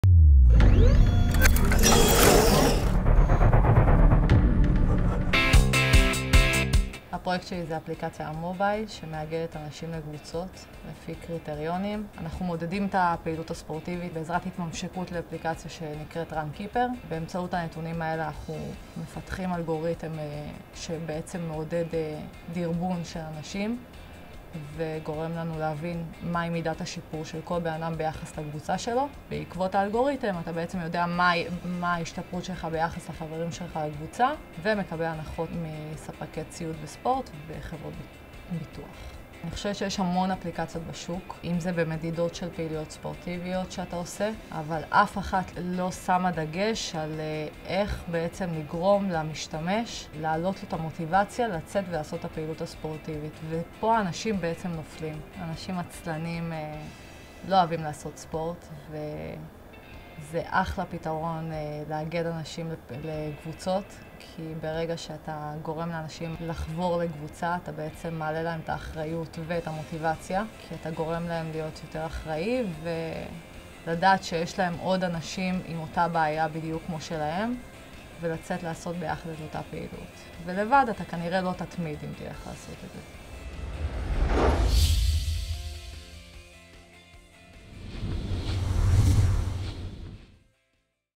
עידוד פעילות ספורט בקבוצות
נופר לגזיאל, בהנחיית דוד בנימין
המודעות לאורח חיים בריא ולחשיבות הפעילות הספורטיבית מתרחבת בציבור, אבל רבים מתקשים להתמיד. המערכת נועדה לטפל בשורש הבעיה, המוטיבציה, ע"י עידוד פעילות בקבוצות. המערכת תאפשר לכל אדם לבחור מבין ענפי ספורט שונים את הקבוצה המתאימה ולבצע את הפעילות במסגרת קבוצתית. המערכת תעודד ותתגמל עפ"י התמדה והישגים ובכך תתרום לשיפור במוטיבציה.